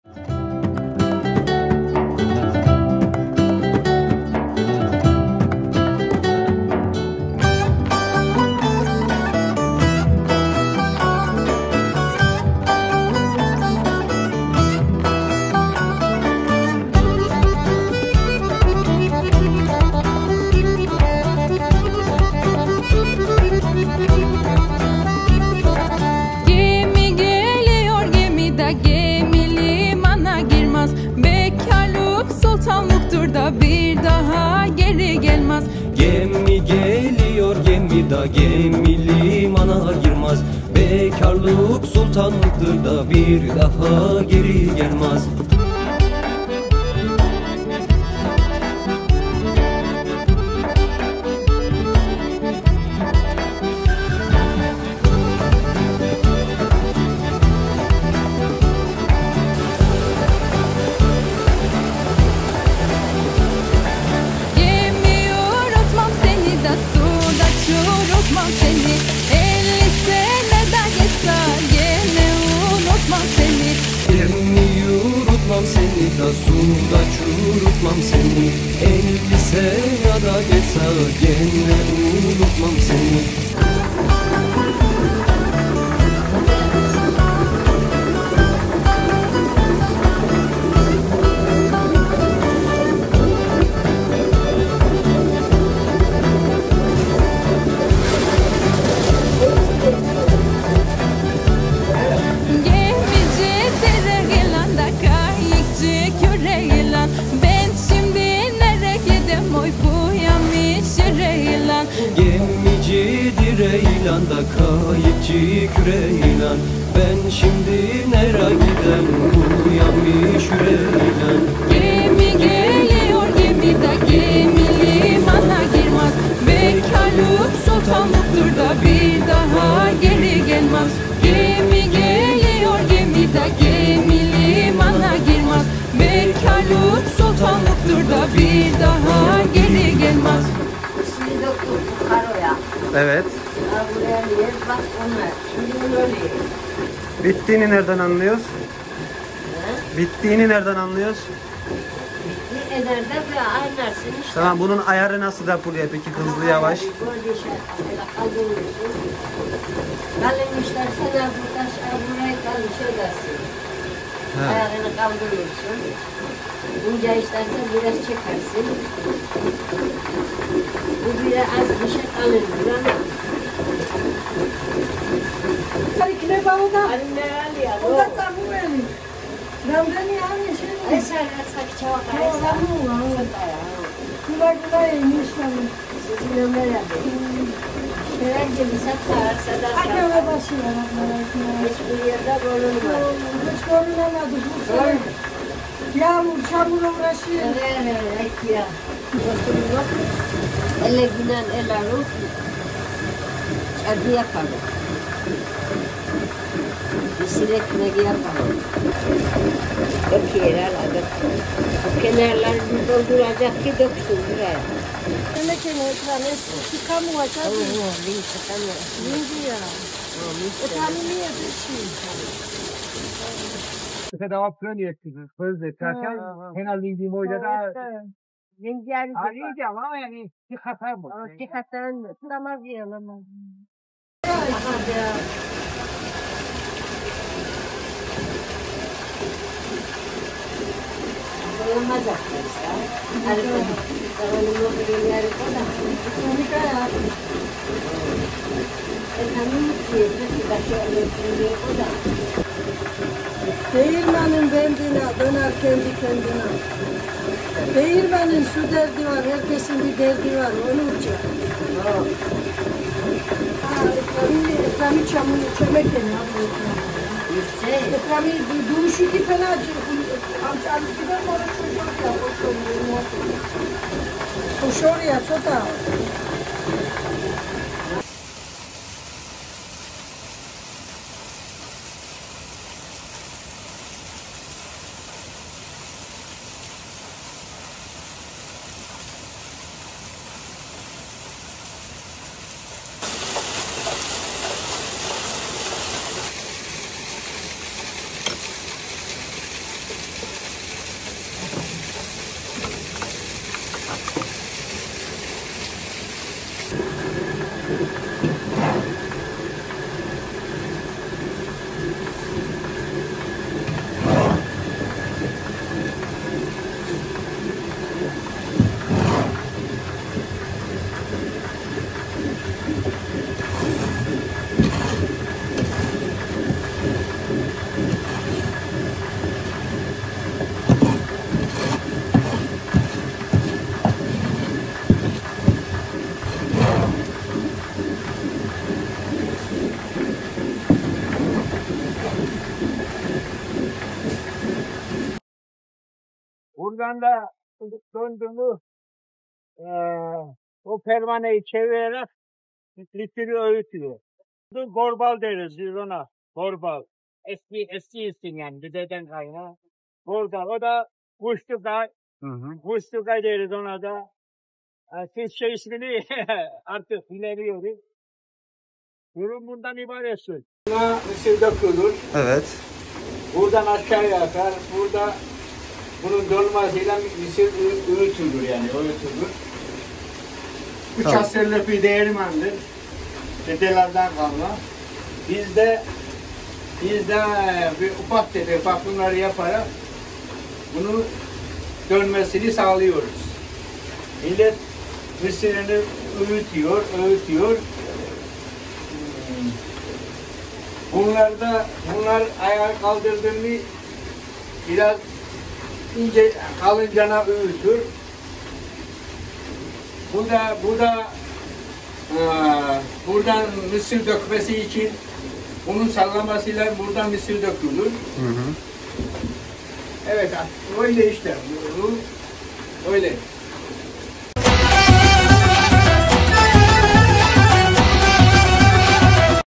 300 yıllık Su değirmeni | Mısırın Serüveni
Ata tohumu mısırlarımızı su değirmeninde öğüttük kışa hazırlanıyoruz işte su değirmeninde geçirdiğimiz o anlar